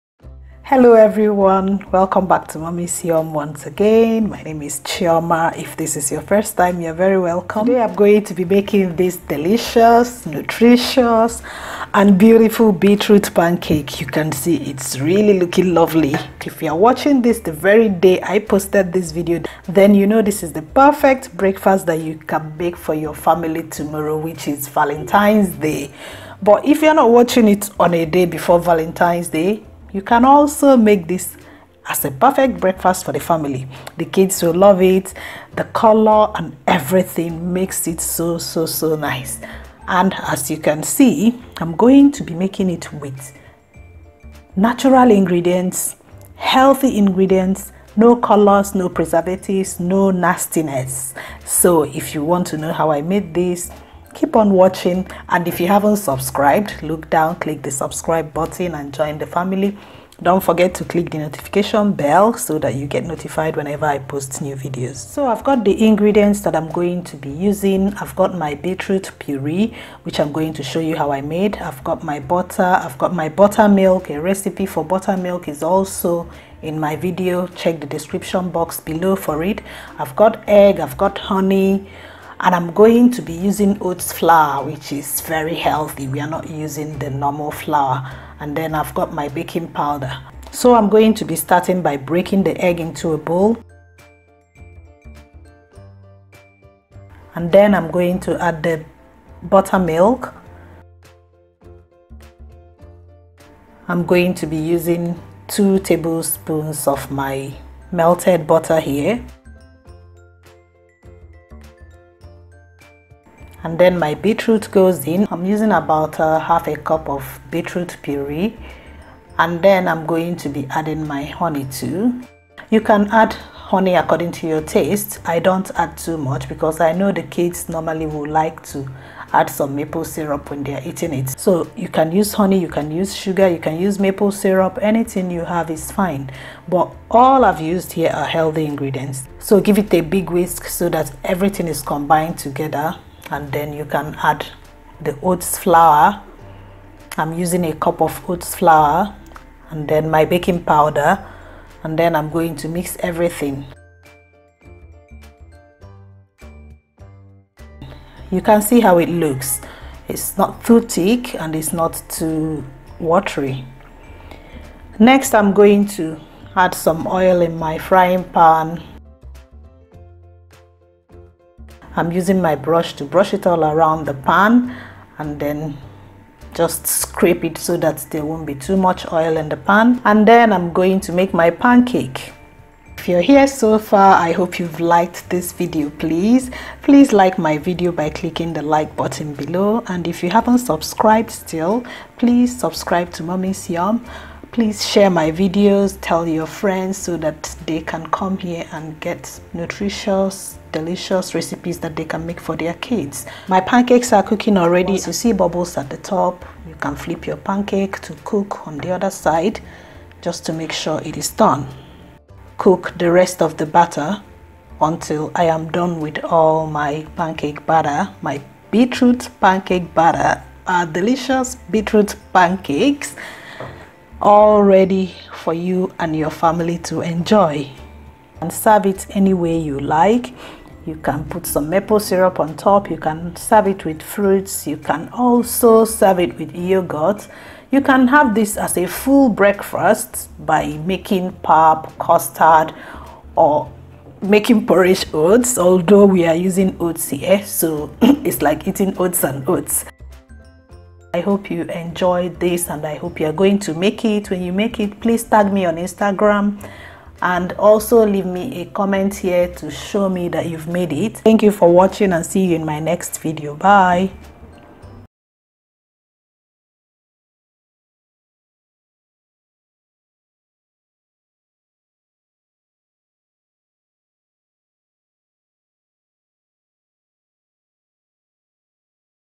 OATS & BEETROOT PANCAKE | HEALTHY BEETS PANCAKE
I hope you'll enjoy this beautiful beetroot pancake breakfast.

Ingredients:
Egg
Melted butter
Honey or any sweetener
Baking powder

YOUR FAVOURITE THINGS IN USA
YOUR FAVOURITE THINGS IN UK